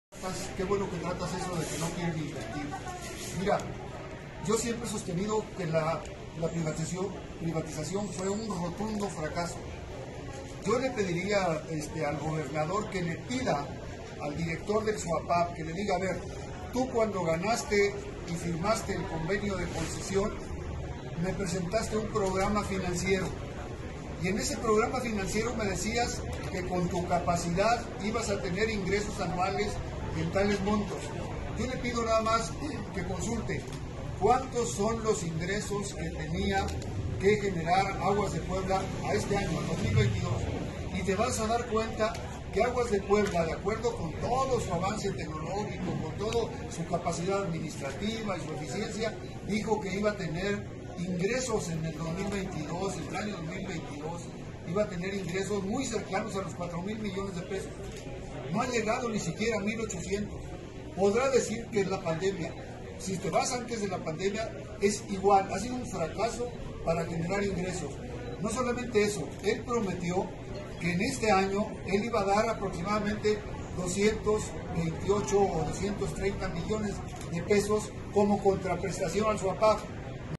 Agua de Puebla dijo que en 2022 dijo que tendría ingresos por 4 mil mdp; no llega a mil 800: FCM
La empresa Agua de Puebla incumplió su objetivo de recaudación, con el que ganó la concesión. En 2022 dijo que tendría ingresos por 4 mil mdp; ahora no llega a mil 800, critica Francisco Castillo